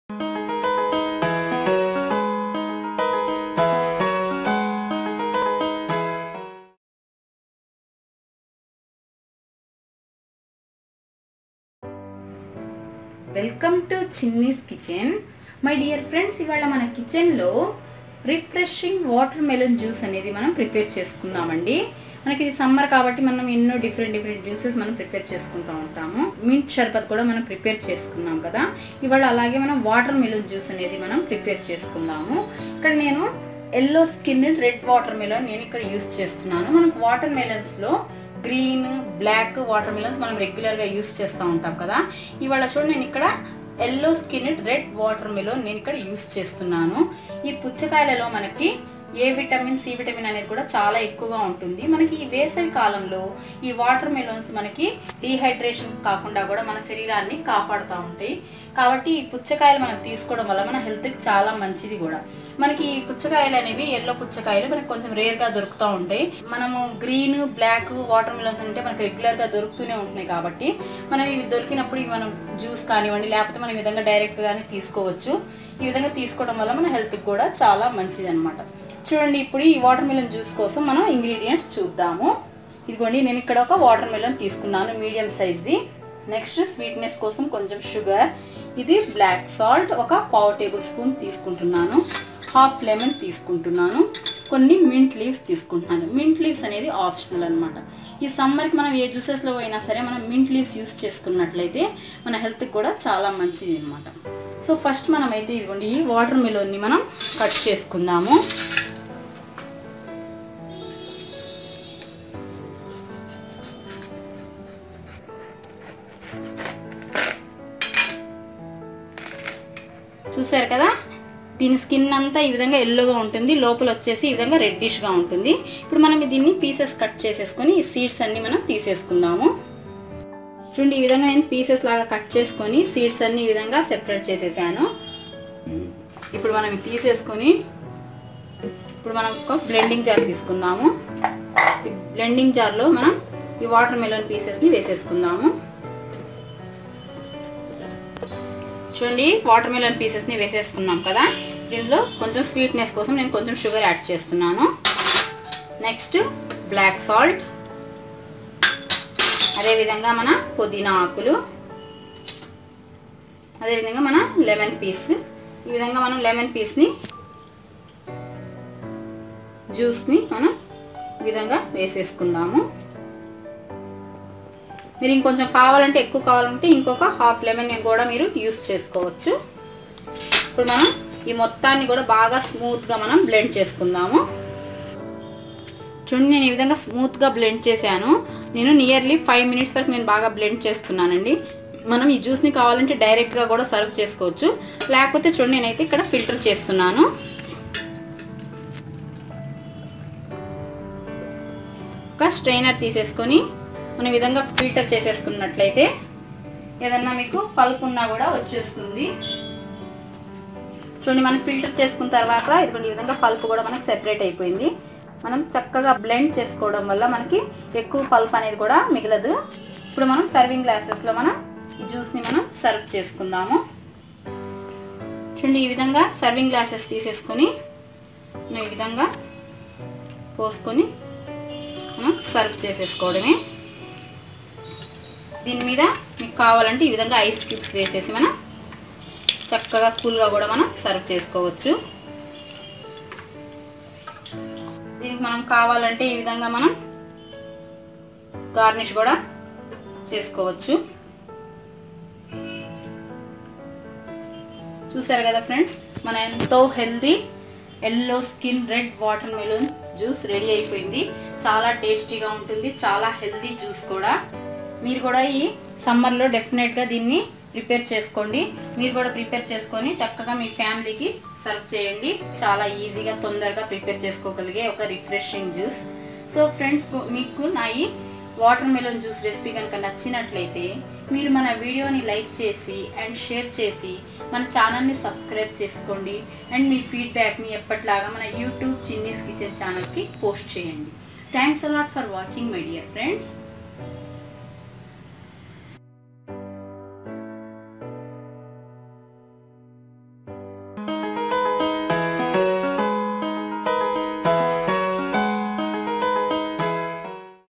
QUICK AND EASY REFRESHING YELLOW SKIN RED WATERMELON JUICE// तरबूज की शरबत// చల్లటి పుచ్చకాయ రసం
Watermelon pieces, black salt-1/4tbs, mint leaves, sugar-as per our taste,lemonjuice.